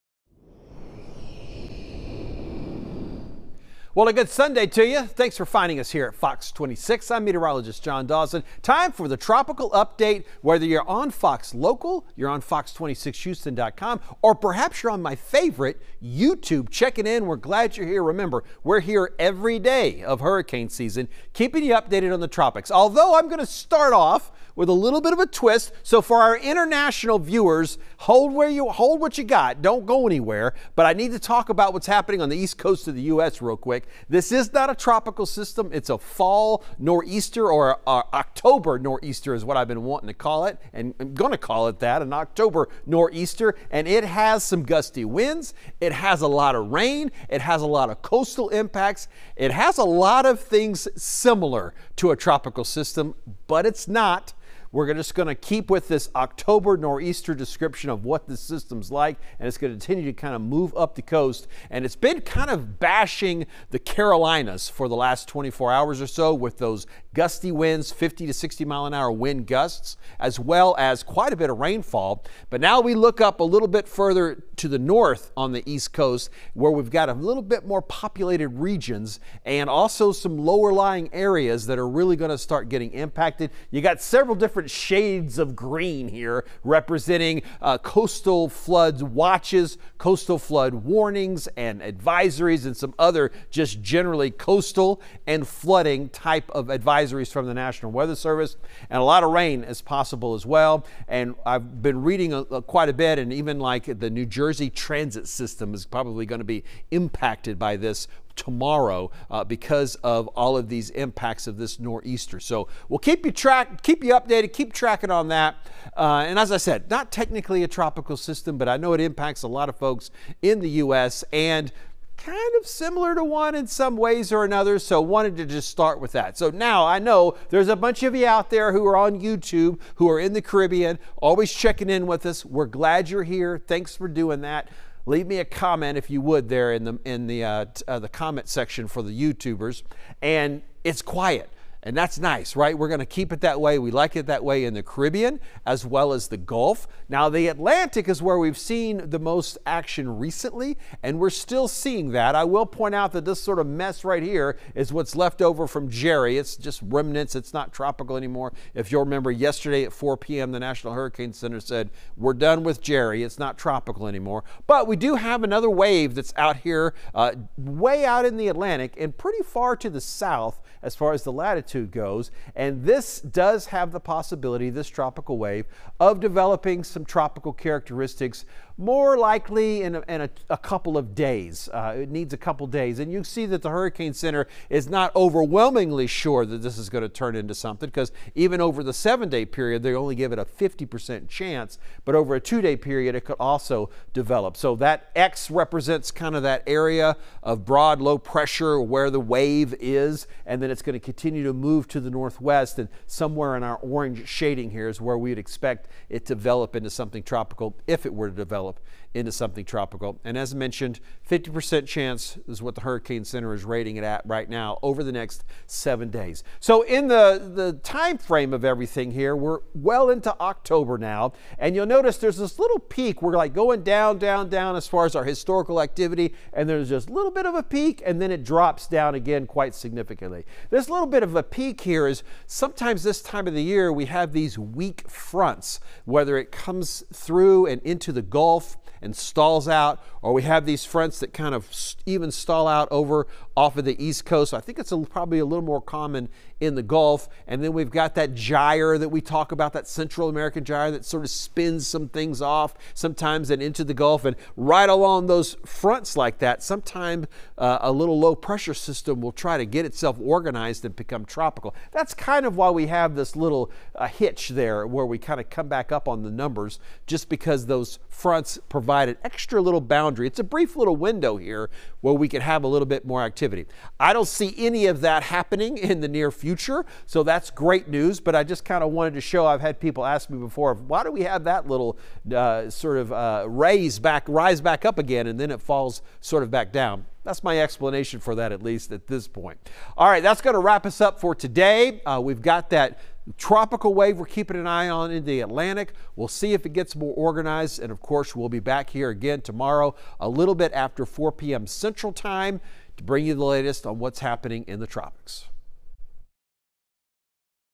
Tropical Weather Update - October 12, 2025
An area of low pressure in the central tropical Atlantic has now been identified as Invest 97L by the National Hurricane Center, located well west-southwest of the Cabo Verde Islands continues to produce a large area of disorganized showers and thunderstorms. Environmental conditions appear conducive for some development of this system during the next few days, and a tropical depression could form during the early or middle part of this week while it moves across the central tropical Atlantic.